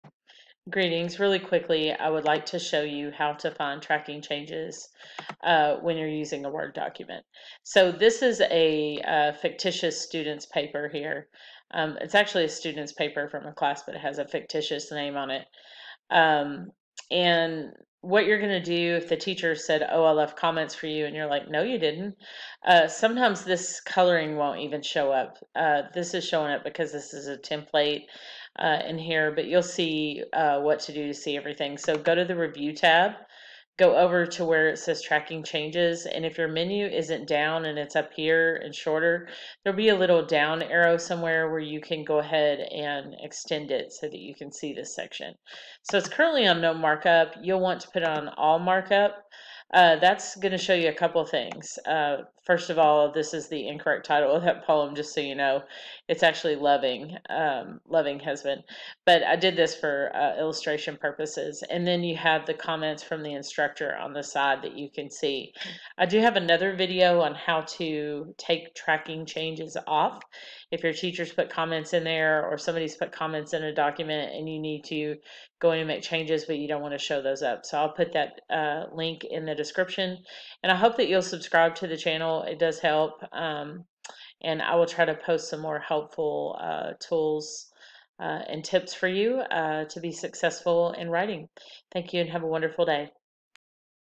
How to See Full Comments in a Word Document - track changes
This video shows you how to see comments made inside of a Word document (if there are any). For example, you may need this information to see comments made by an instructor that you can't see/see limited view.